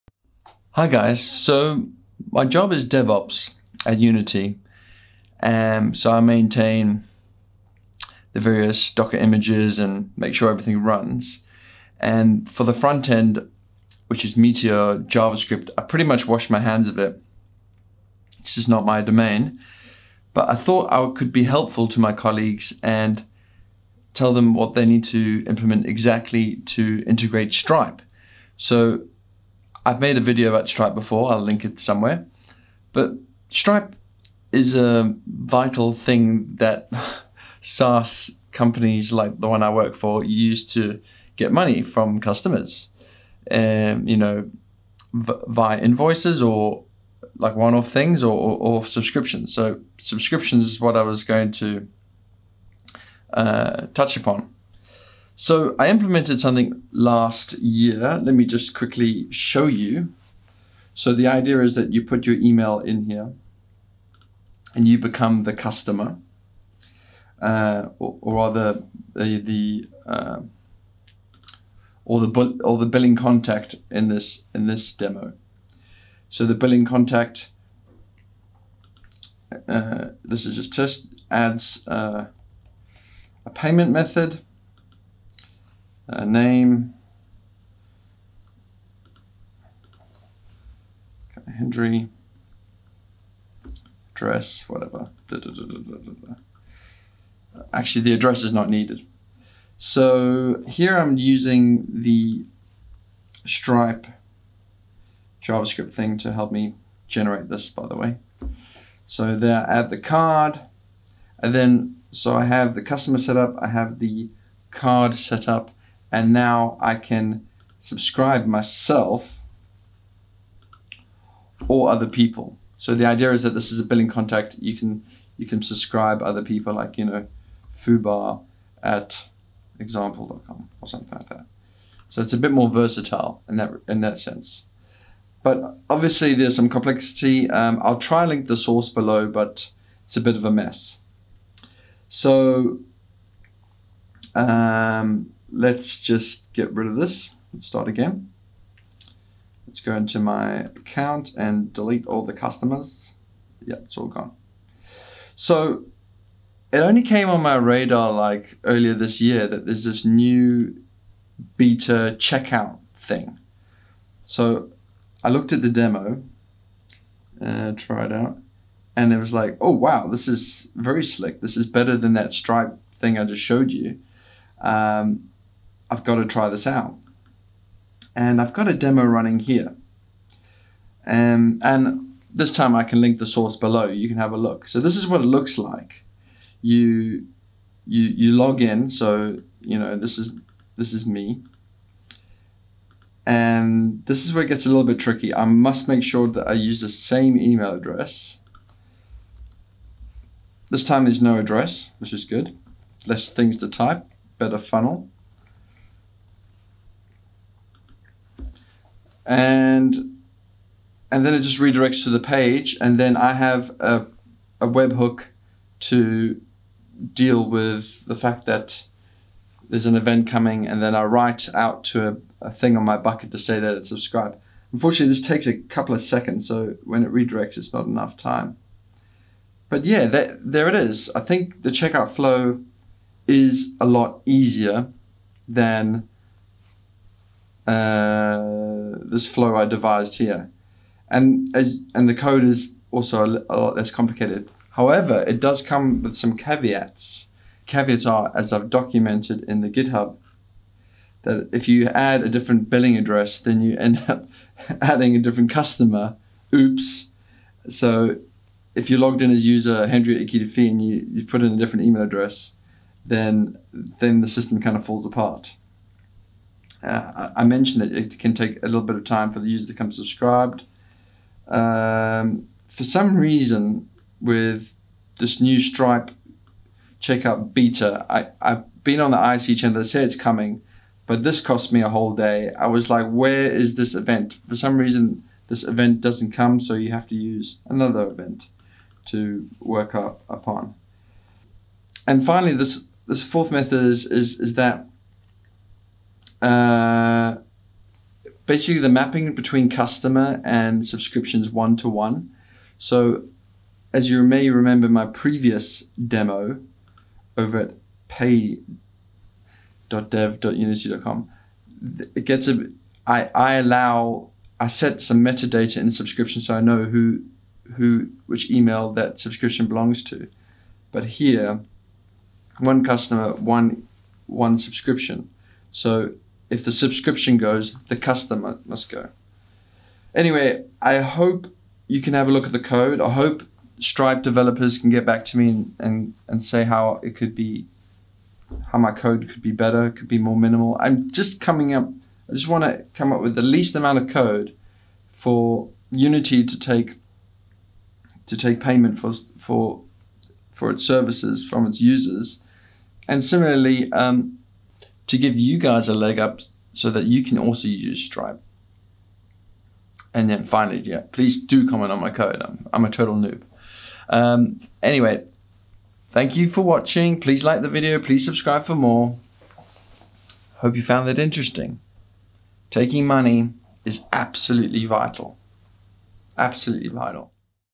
Stripe checkout BETA impressions
is my Golang source for a minimal implementation

Could it be simpler?